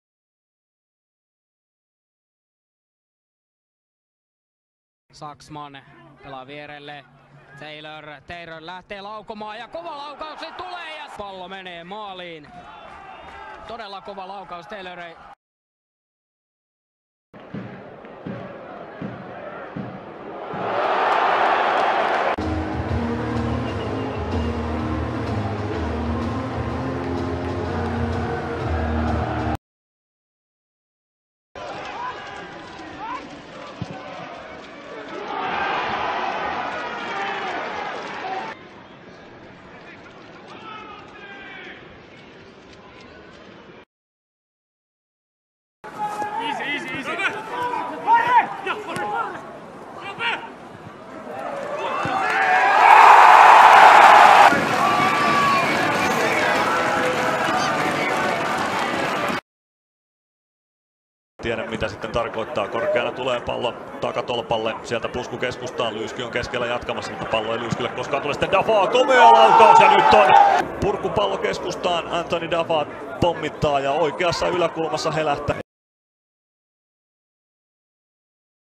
Veikkausliiga Top-5 goals in April 2016
The best goals in the Finnish Veikkausliiga April 2016